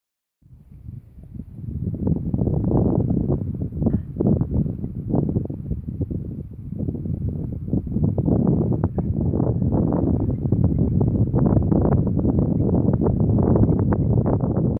Mkhombo's Mystery Gull
The long-staying Lesser Black-backed Gull at Mkhombo Dam, northern South Africa. This bird caused some excitement because of its very pale grey plumage, with suggestions of Caspian Gull. It is mostly likely a Steppe Gull, Larus (fuscus) barabensis judging by the plumage, moult progression, shape and bill colour. This is digiscoped footage - apologies for the shake and wind noise.